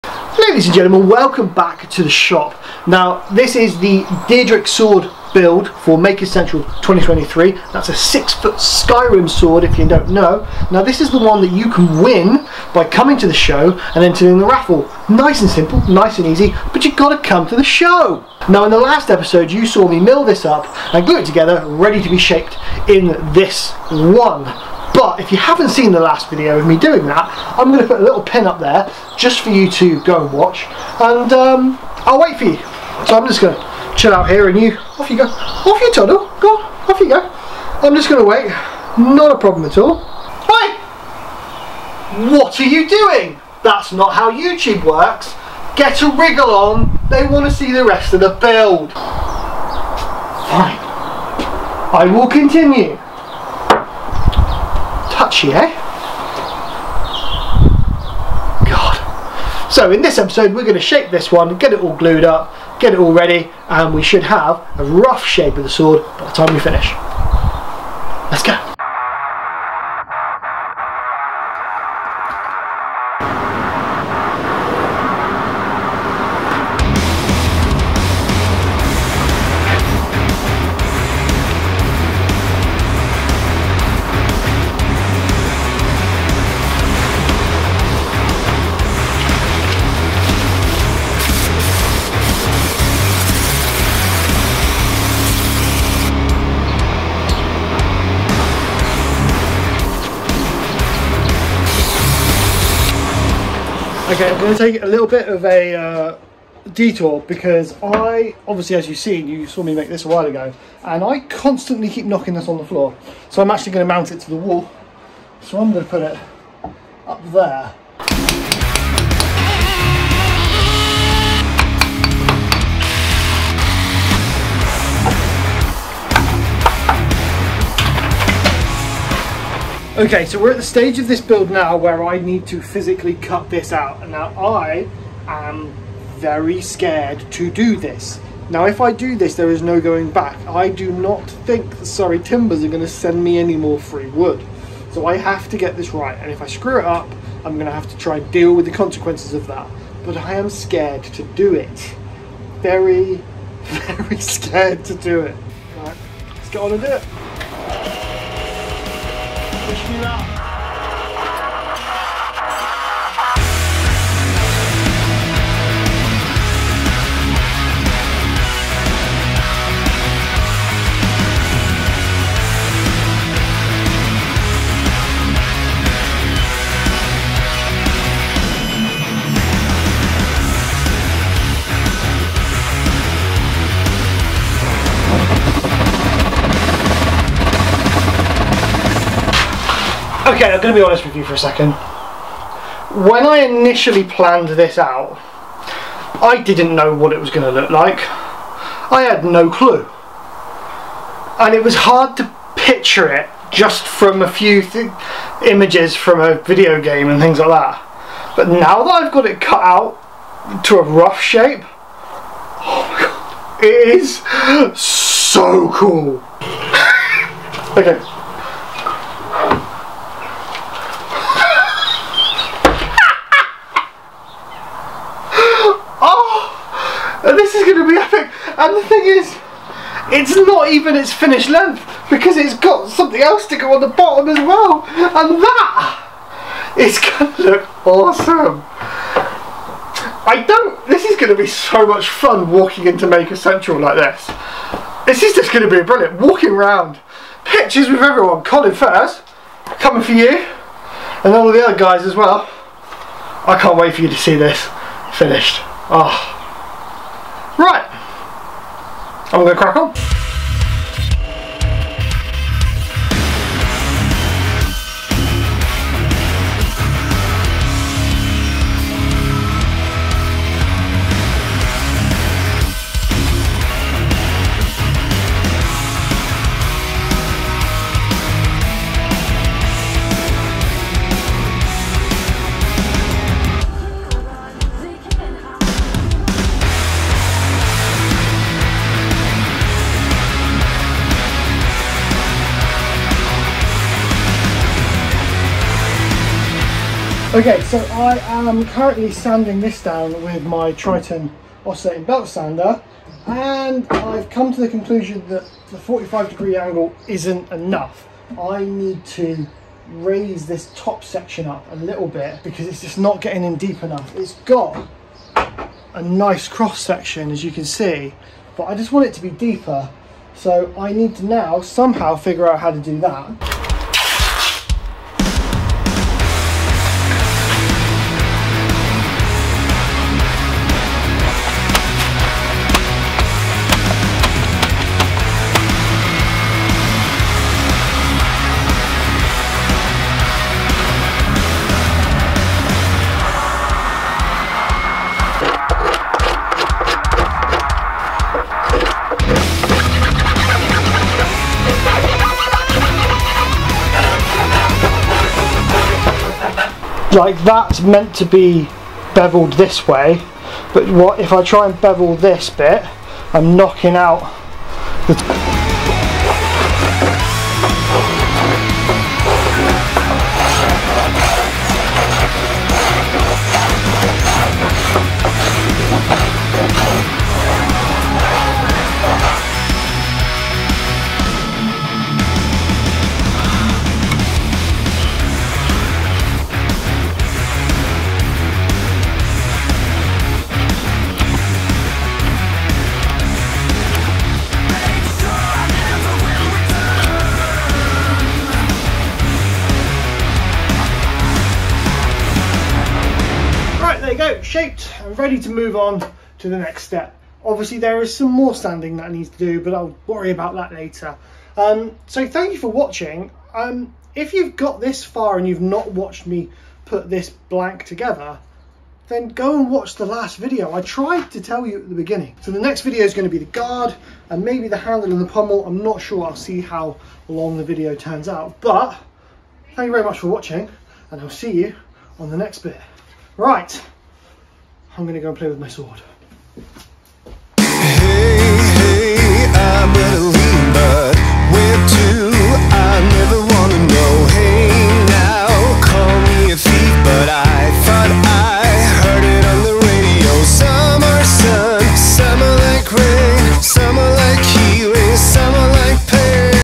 Making a GIANT Skyrim Great Sword Part 2
Making a GIANT Deadric great sword from the Edler Scrolls Skyrim game. This is for the annual  @makerscentral  event. I make a GIANT weapon every year to give away.

Get tickets for the show via this link & I will see you there
A massive thank you to  @nickzammeti  & everyone for making this event happen.